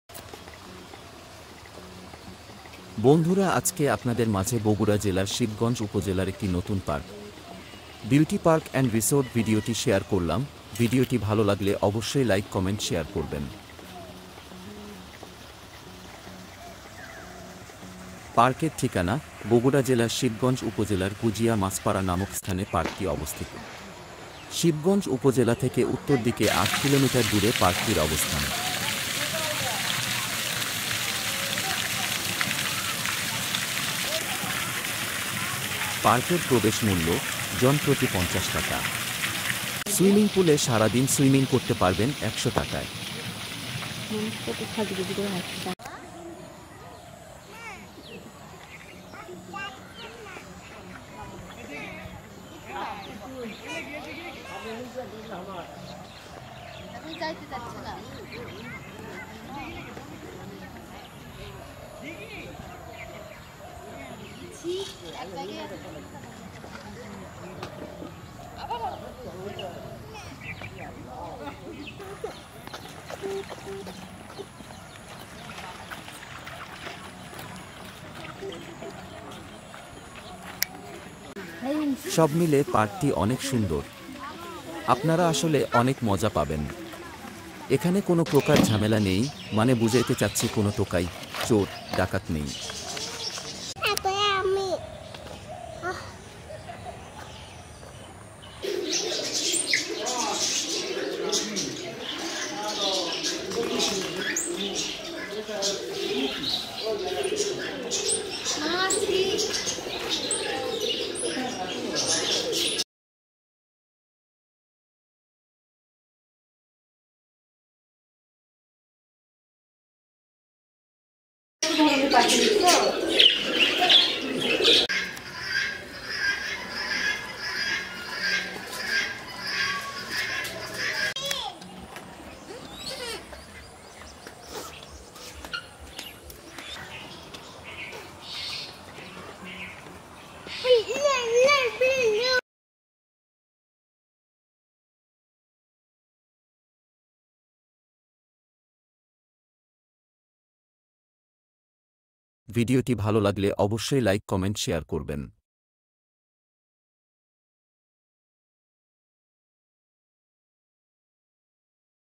বাংলাদেশের নতুন পার্ক - পিকনিক স্পট - বগুড়া বিউটি পার্ক এন্ড রিসোর্ট | Beauty Park & Resort Bogura
পার্কের পুরো  ঠিকানা:
জেলার: বগুড়া,
উপজেলা: শিবগঞ্জ,
গ্রাম/পাড়া: গুজিয়া মাছপাড়া

শিবগঞ্জ থেকে উত্তর দিকে  ৭-৮ কিলোমিটার দূরে পার্কটির অবস্থান।

পার্কের প্রবেশ মূল্য: জন প্রতি পঞ্চাশ (৫০)টাকা।

সুইমিংপুলে সারাদিন সুইমিং করতে পারবেন একশো (১০০) টাকায়